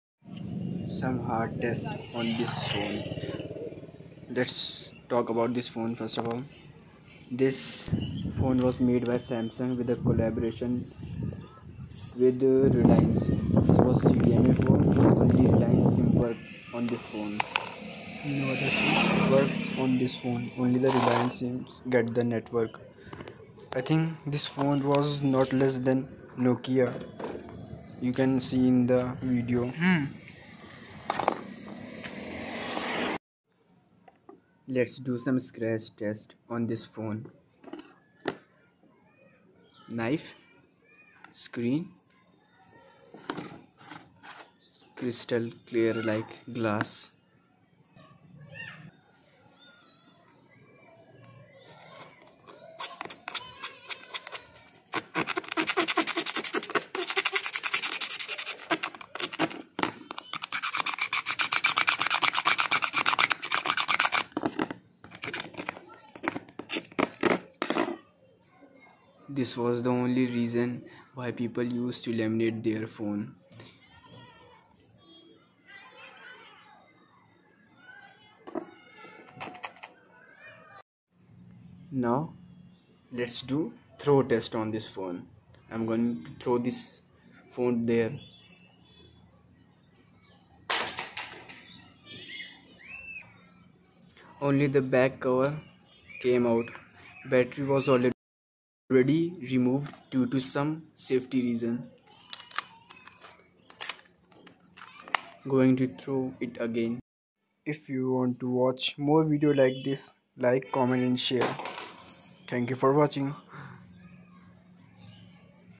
Samsung Reliance mobile phone under bike P2| Durability test of samsung mobile | stronger than Nokia
Samsung Reliance mobile phone under bike P2| Durability test of samsung mobile | stronger than Nokia || It's Nokia